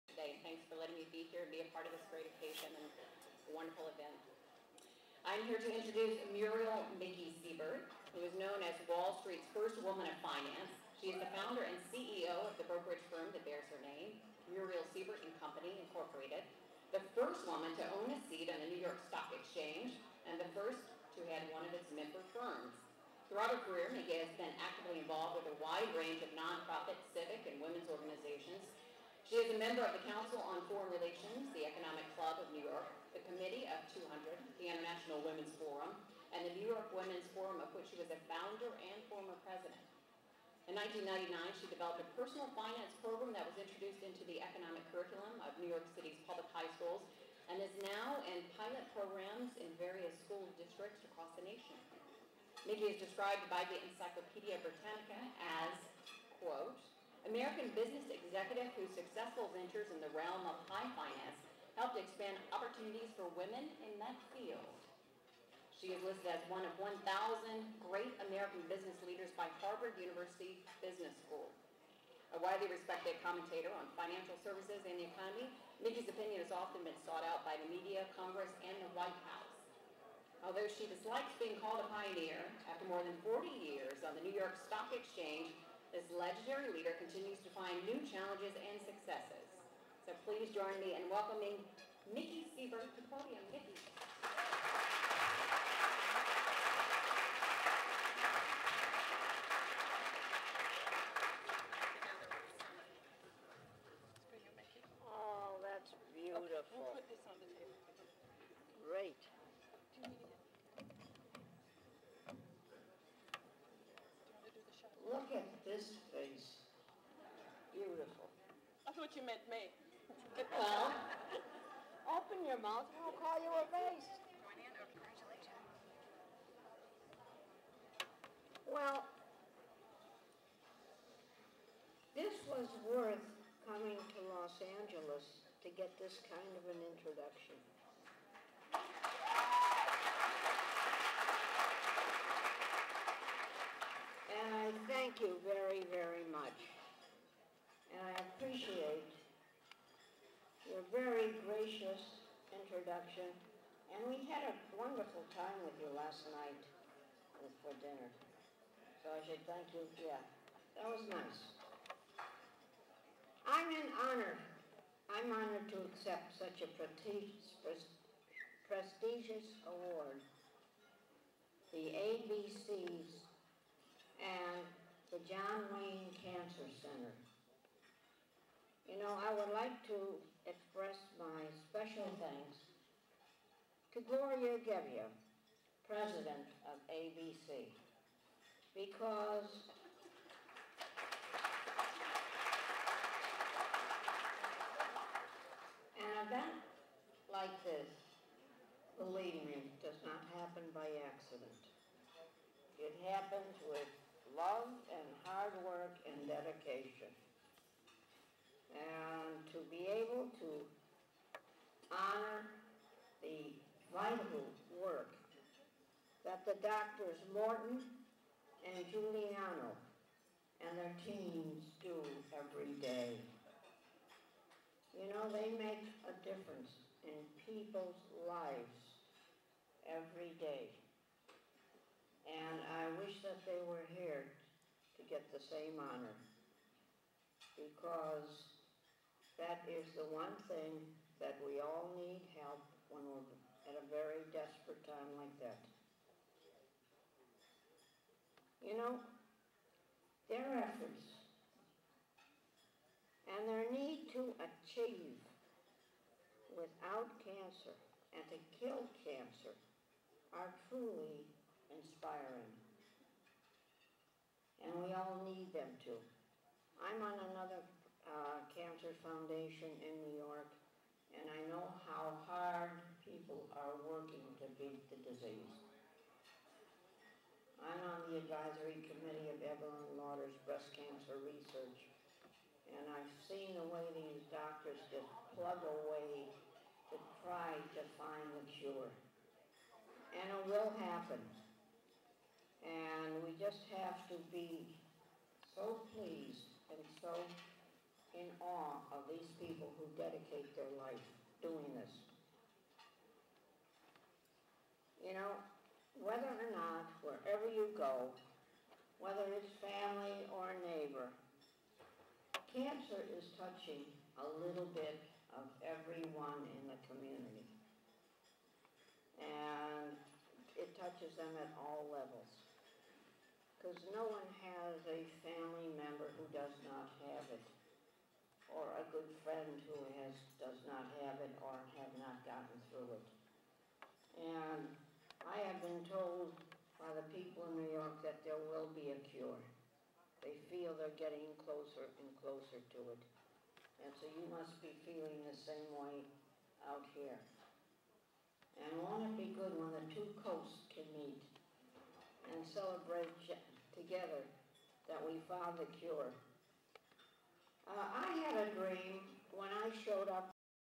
Muriel Siebert ABC Mothers Day Luncheon 2006
ABC's President Gloria Gebbia, honors Muriel Siebert, at the 2006 Mothers Day Luncheon.